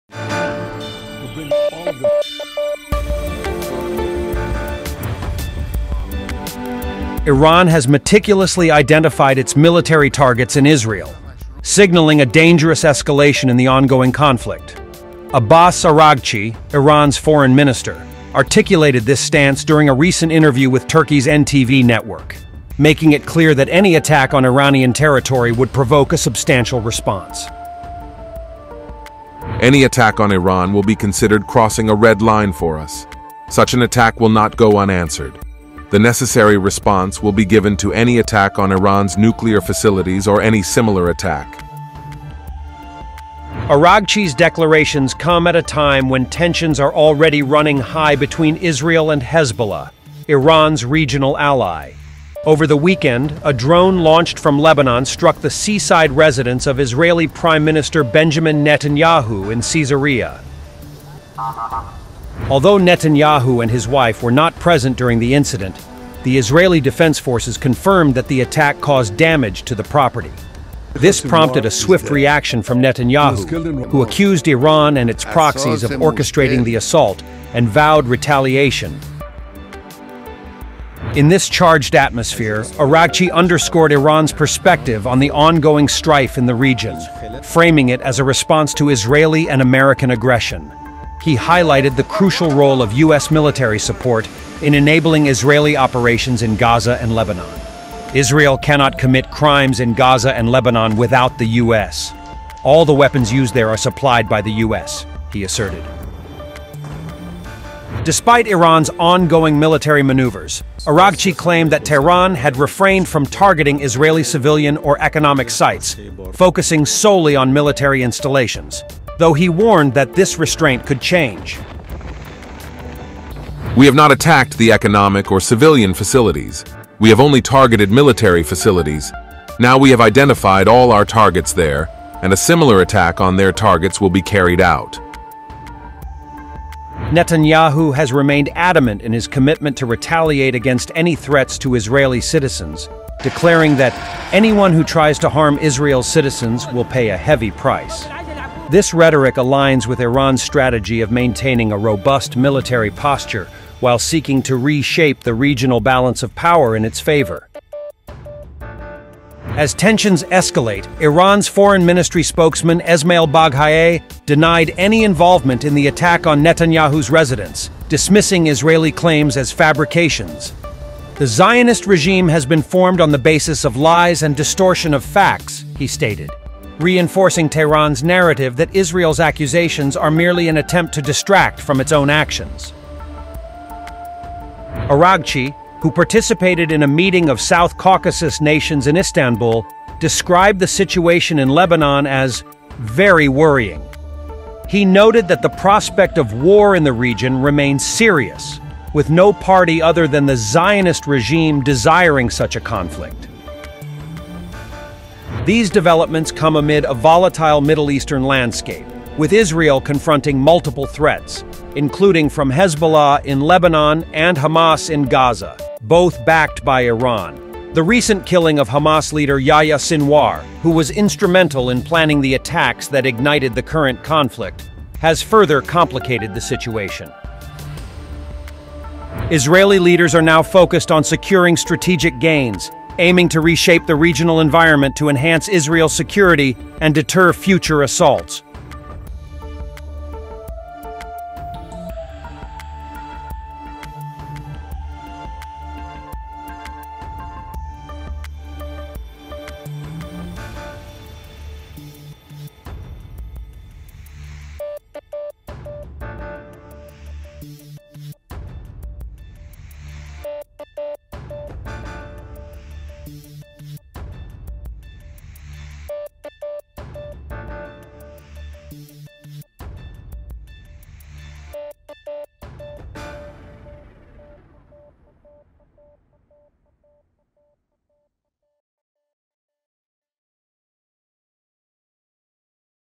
Middle East on the Brink: Iran’s Military Prepares to Strike as Israeli Tensions Explode| ET NOW
Iran warns of swift retaliation if its nuclear sites are attacked, following rising tensions with Israel after a drone strike on Netanyahu's residence. Both nations prepare for potential conflict amid escalating threats. Watch to know more.